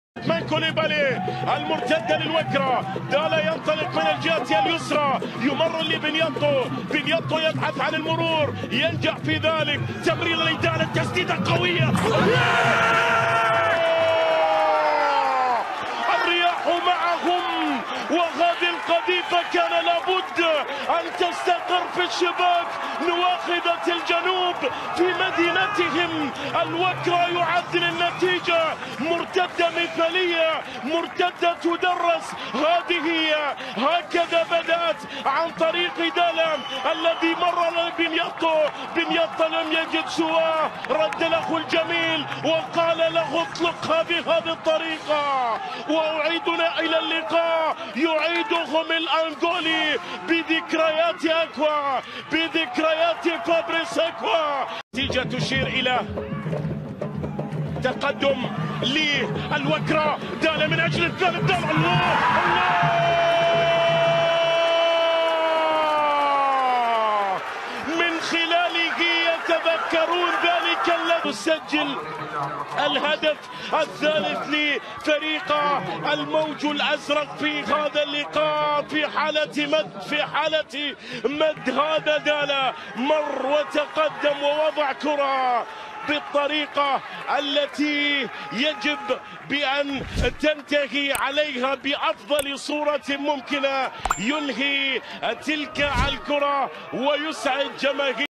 Gelson Dala faz dois golaços e ganha aplausos
Canal D17